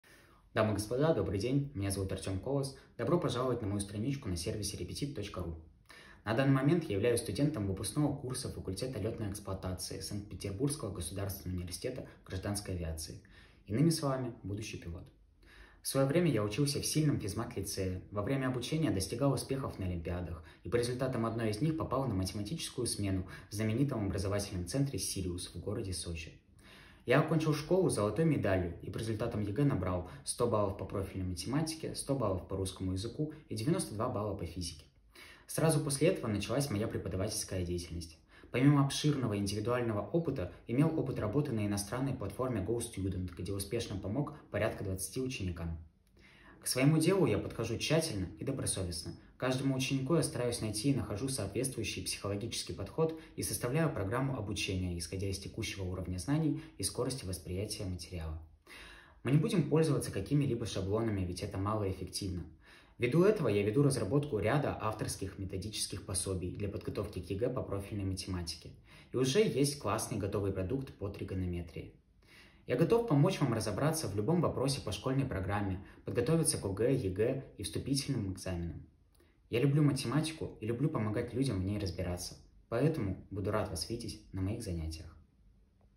Колос Артем Владимирович - репетитор по математике - видеопрезентация для Repetit.ru
Видеопрезентация репетитора по математике Колоса Артема Владимировича для REPETIT.RU.

Колос Артем Владимирович студент Санкт-Петербургского государственного университета гражданской авиации.